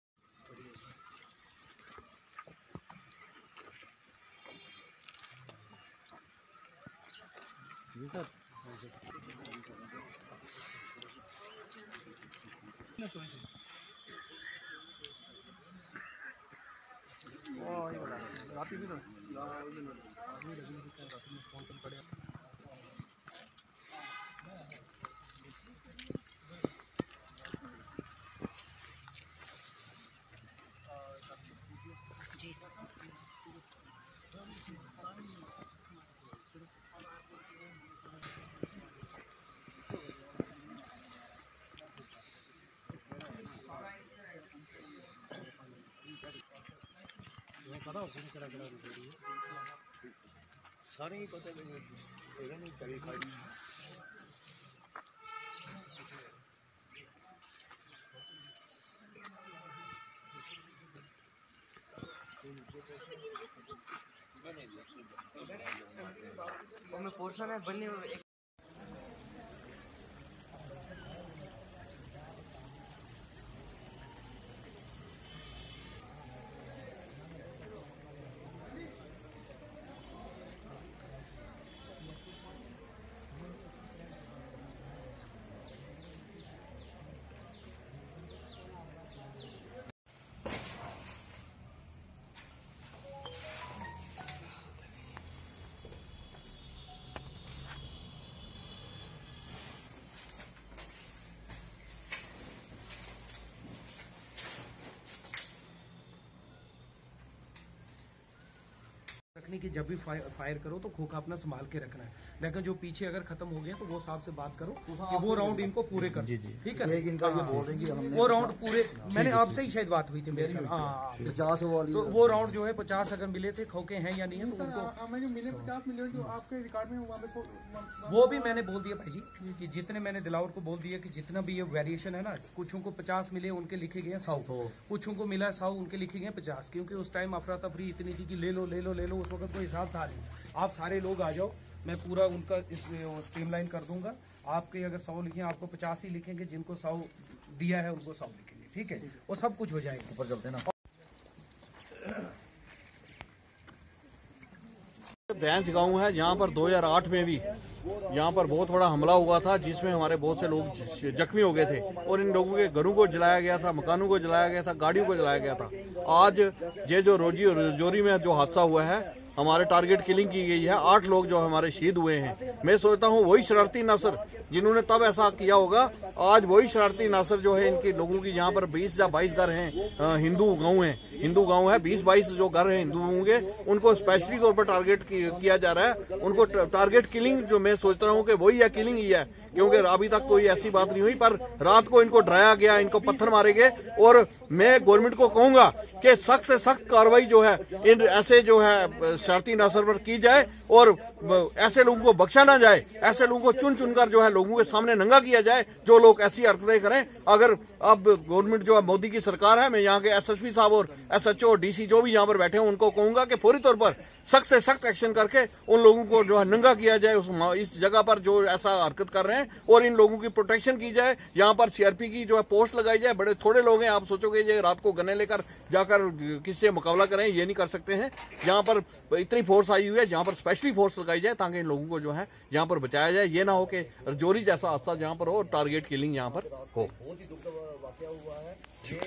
Incident of stone pelting at the houses of minorities in Bhench village near Poonch town
Incident of stone pelting at the houses of minorities in Bhench village near Poonch town during night.